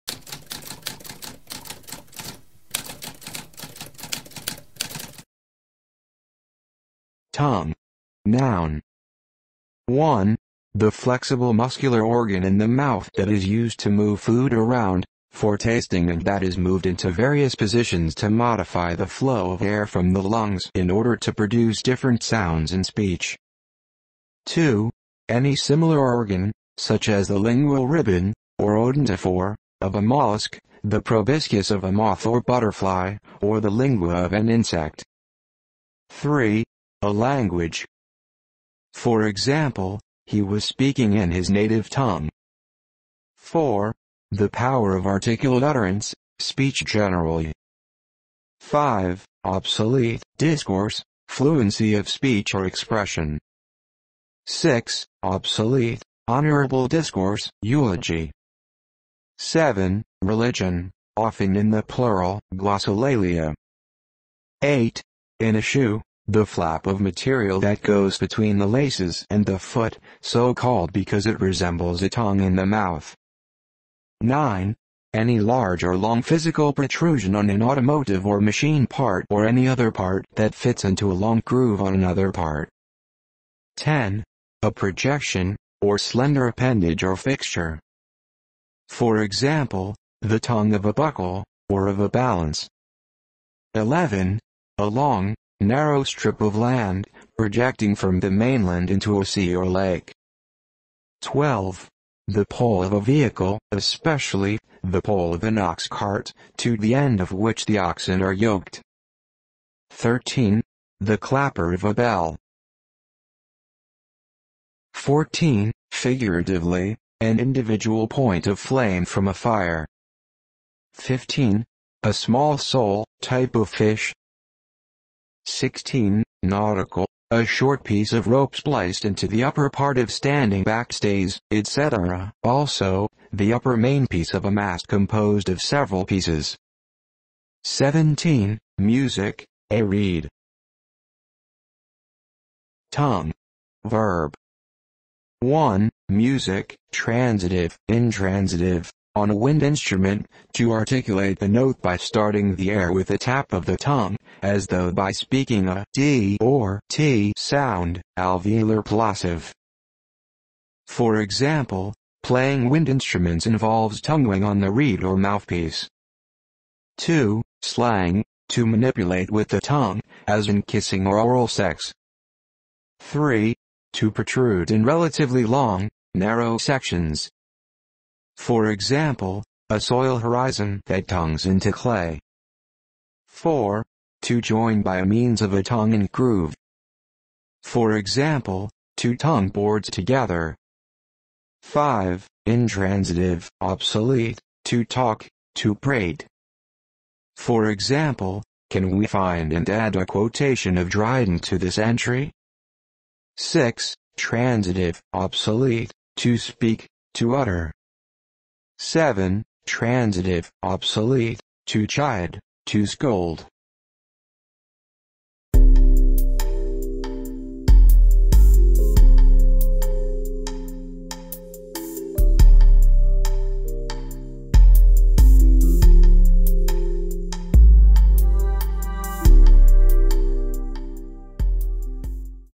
What does tongue mean?
A spoken definition of tongue.

Intro Sound:
Typewriter - Tamskp